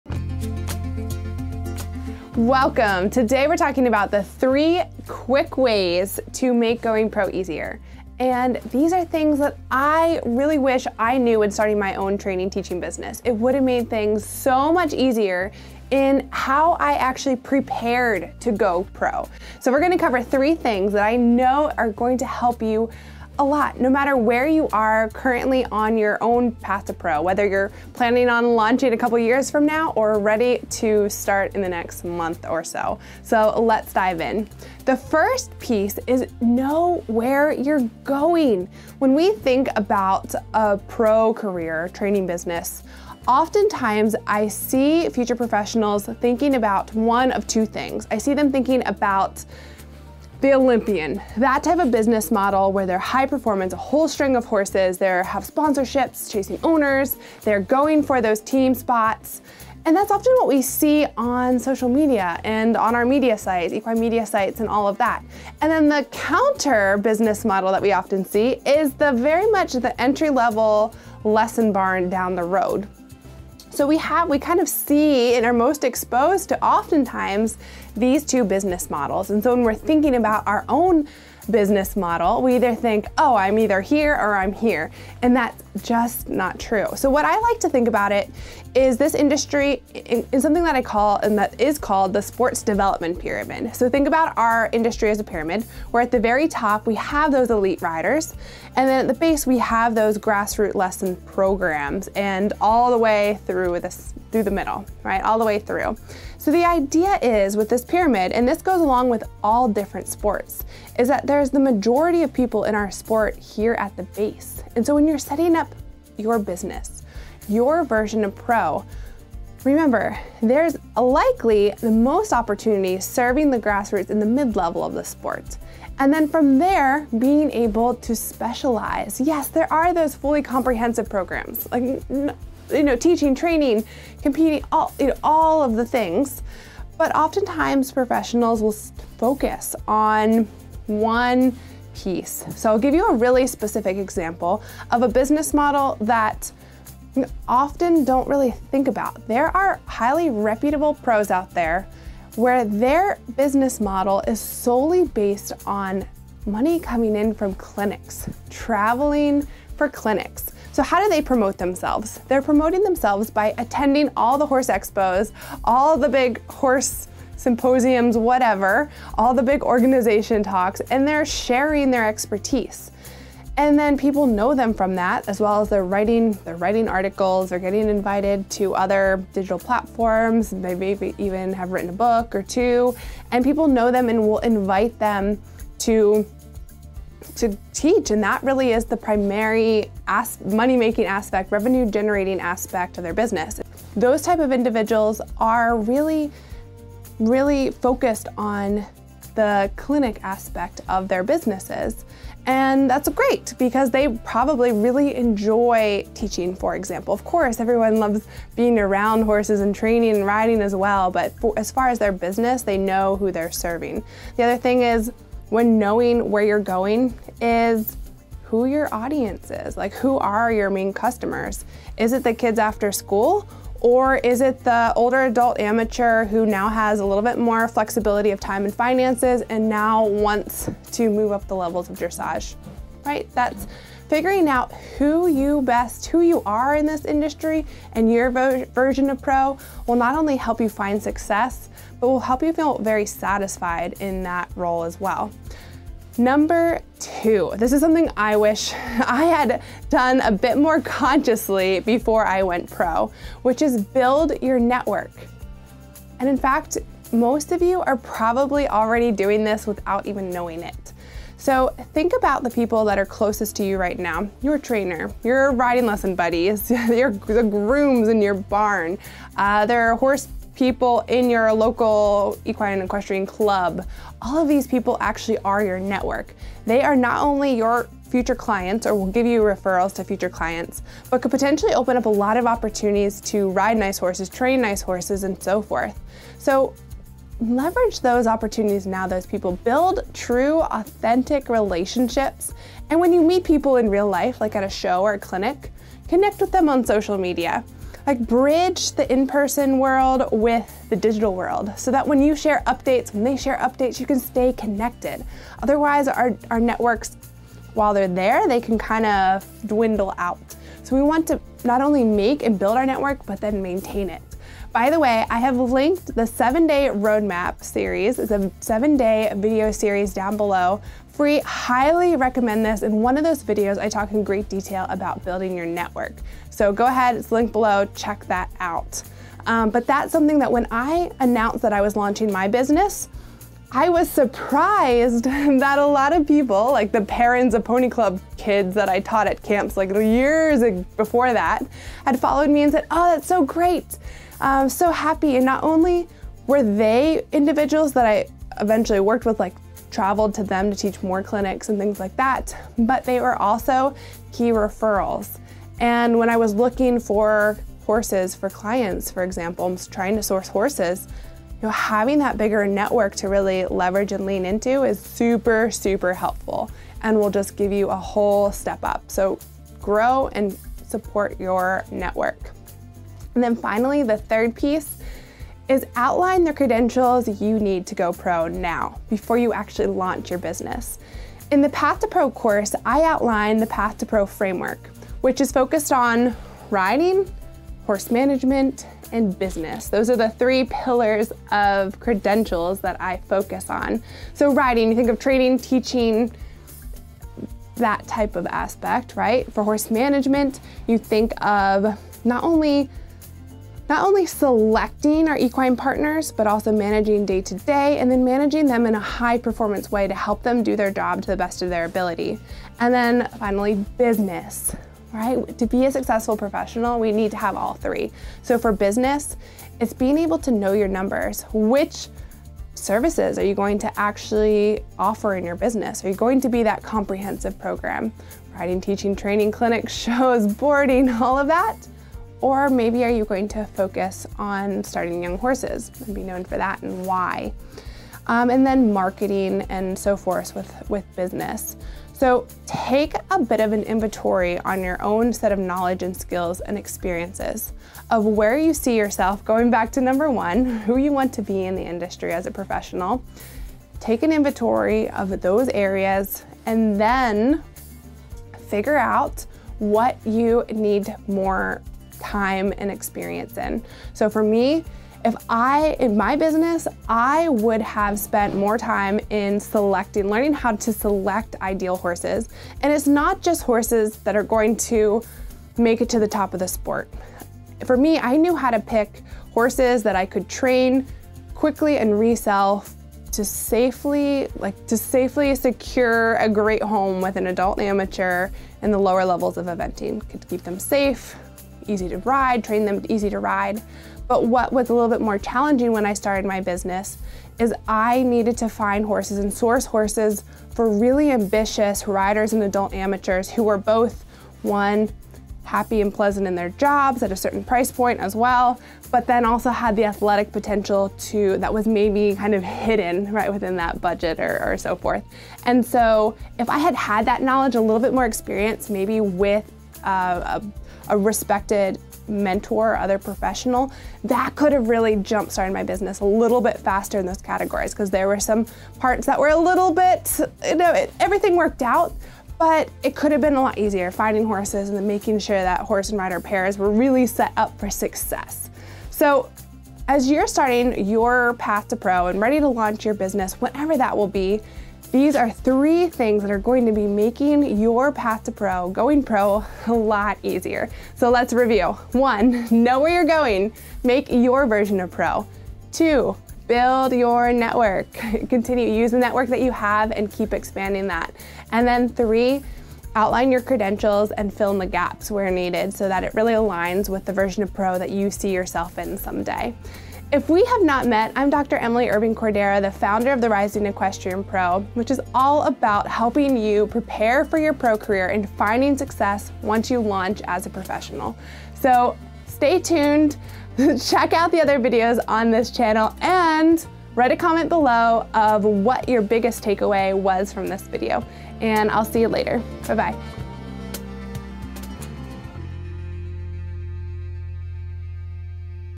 3 Quick Ways to Make Going Pro Easier
Establishing yourself in the equine industry as an equine professional can be overwhelming. I know. Access the FREE 7 Day Roadmap to Pro to get the ‘must knows’ for your path to pro

In this video, I outline 3 ways to make going pro easier. These are things that I wished I knew earlier! They include knowing where you are going, building your network, and outlining your credentials.

Cheers to chasing the dream,
Emily